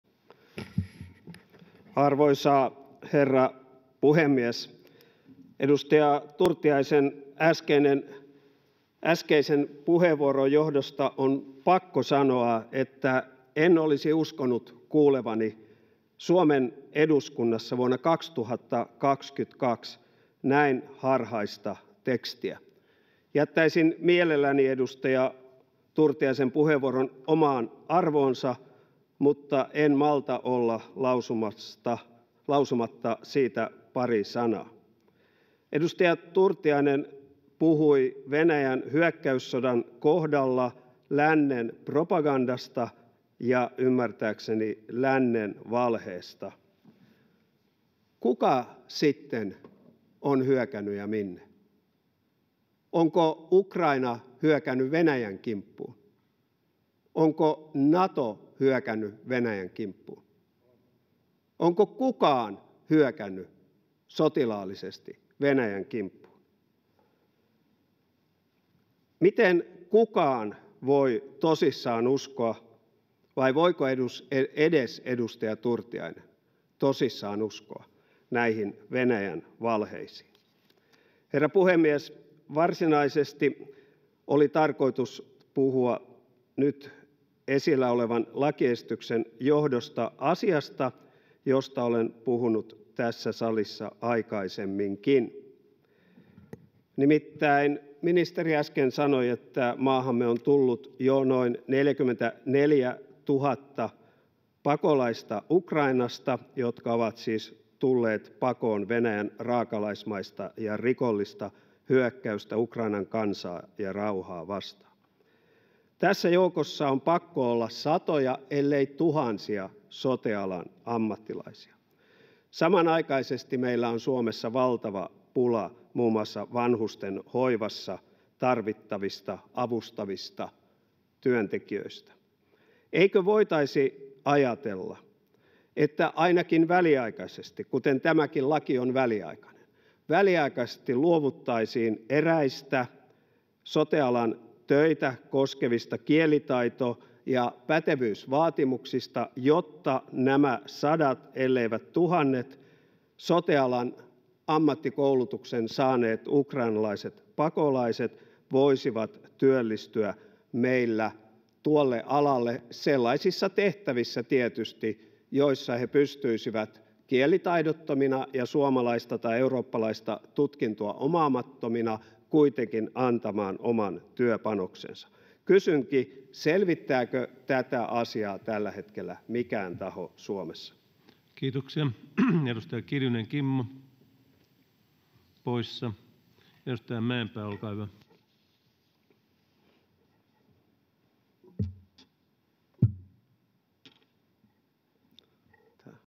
Ben Zyskowicz kokoomus Ano Turtiaisen harhainen teksti
Ben Zyskowicz kok :
Arvoisa herra puhemies! Edustaja Turtiaisen äskeisen puheenvuoron johdosta on pakko sanoa, että en olisi uskonut kuulevani Suomen eduskunnassa vuonna 2022 näin harhaista tekstiä. Jättäisin mielelläni edustaja Turtiaisen puheenvuoron omaan arvoonsa, mutta en malta olla lausumatta siitä pari sanaa: Edustaja Turtiainen puhui Venäjän hyökkäyssodan kohdalla ”lännen propagandasta” ja ymmärtääkseni ”lännen valheesta”. Kuka sitten on hyökännyt ja minne? Onko Ukraina hyökännyt Venäjän kimppuun? Onko Nato hyökännyt Venäjän kimppuun? Onko kukaan hyökännyt sotilaallisesti Venäjän kimppuun? Miten kukaan voi tosissaan uskoa — vai voiko edes edustaja Turtiainen tosissaan uskoa — näihin Venäjän valheisiin?

Herra puhemies! Varsinaisesti oli tarkoitus puhua nyt esillä olevan lakiesityksen johdosta asiasta, josta olen puhunut tässä salissa aikaisemminkin: Nimittäin ministeri äsken sanoi, että maahamme on tullut jo noin 44 000 pakolaista Ukrainasta, jotka ovat siis tulleet pakoon Venäjän raakalaismaista ja rikollista hyökkäystä Ukrainan kansaa ja rauhaa vastaan. Tässä joukossa on pakko olla satoja ellei tuhansia sote-alan ammattilaisia. Samanaikaisesti meillä on Suomessa valtava pula muun muassa vanhustenhoivassa tarvittavista avustavista työntekijöistä. Eikö voitaisi ajatella, että ainakin väliaikaisesti, kuten tämäkin laki on väliaikainen, luovuttaisiin eräistä sote-alan töitä koskevista kielitaito- ja pätevyysvaatimuksista, jotta nämä sadat elleivät tuhannet sote-alan ammattikoulutuksen saaneet ukrainalaiset pakolaiset voisivat työllistyä meillä tuolle alalle, sellaisissa tehtävissä tietysti, joissa he pystyisivät kielitaidottomina ja suomalaista tai eurooppalaista tutkintoa omaamattomina kuitenkin antamaan oman työpanoksensa? Kysynkin: selvittääkö tätä asiaa tällä hetkellä mikään taho Suomessa?